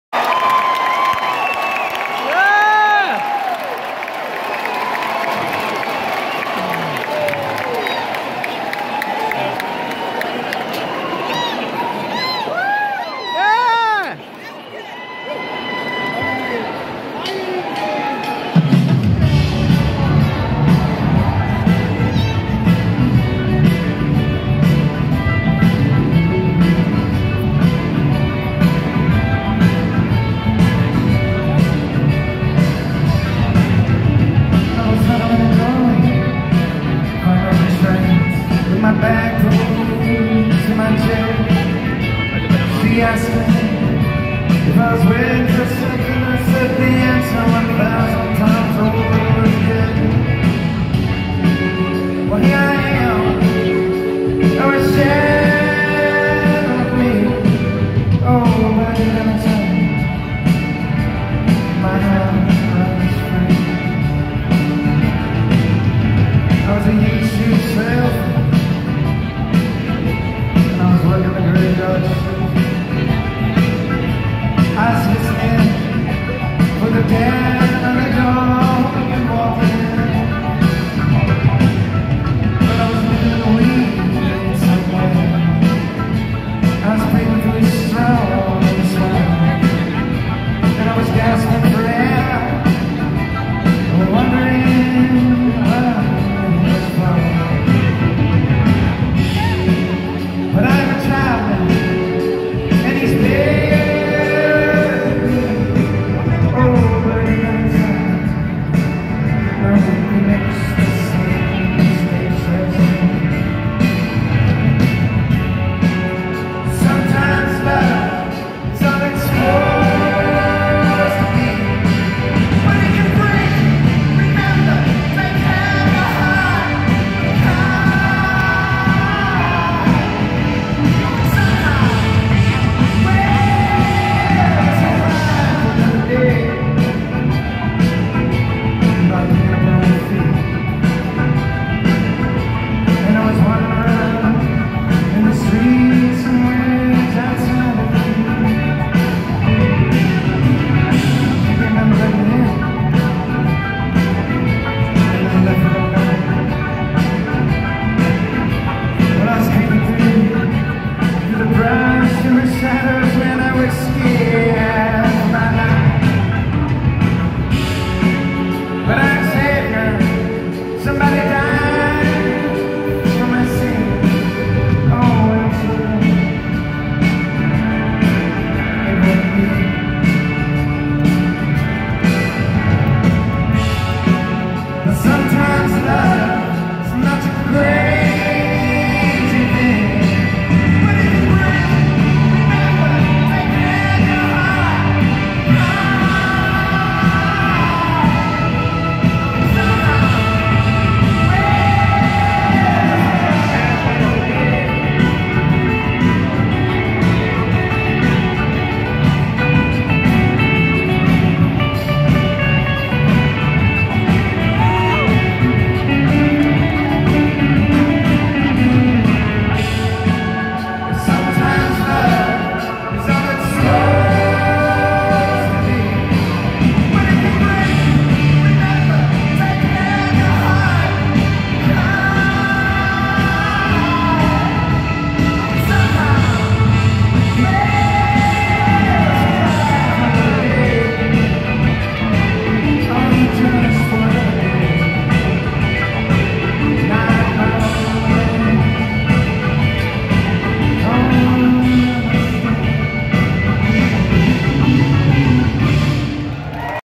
DR DOG AT THE MANN
Dr. Dog perform at The Mann Center in Philly on July 13, 2024